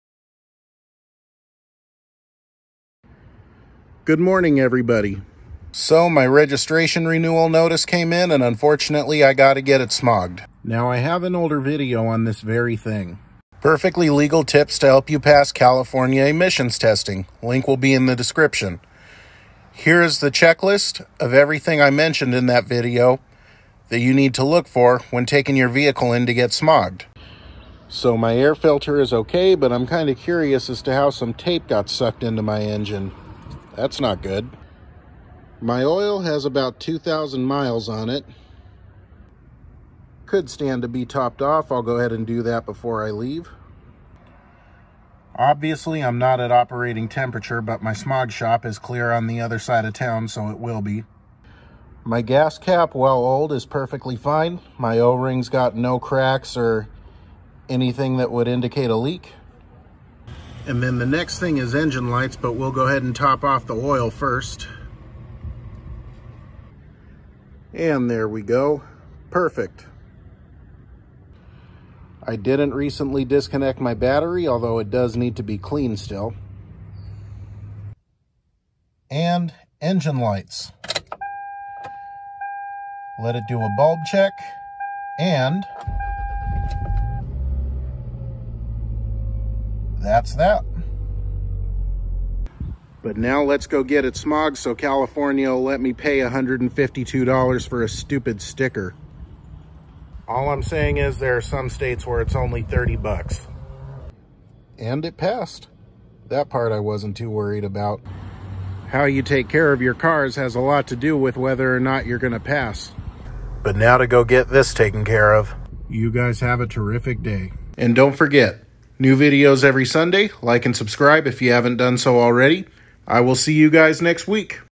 Getting The Highlander Ready For Smog
So my Highlander is due for a smog check.￼ Going to get it ready.

Perfectly Legal Tips To Help You Pass California Emissions Testing